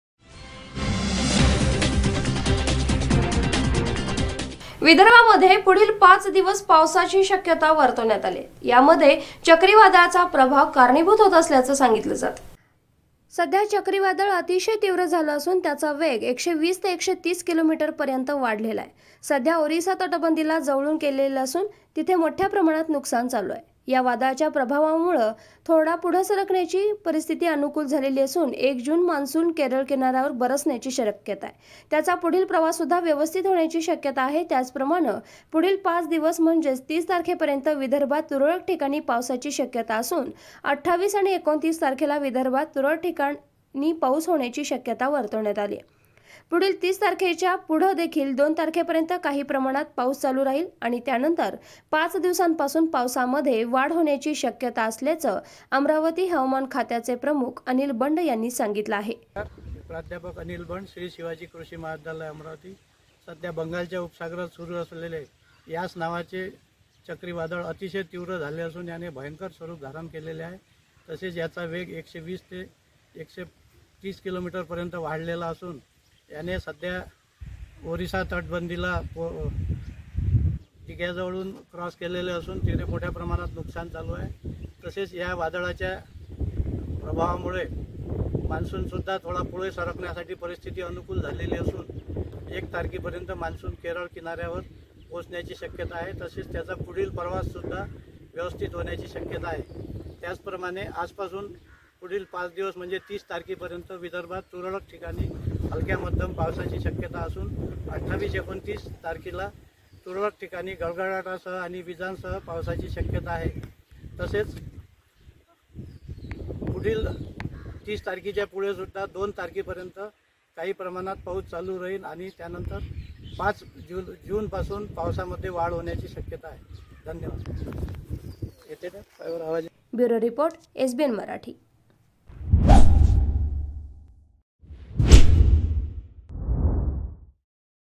MUMBAI | विदर्भात पुढील पाच दिवस पावसाची शक्यता, यास चक्रीवादळाचा प्रभाव
विदर्भात पुढील पाच दिवस पावसाची शक्यता, यास चक्रीवादळाचा प्रभाव.@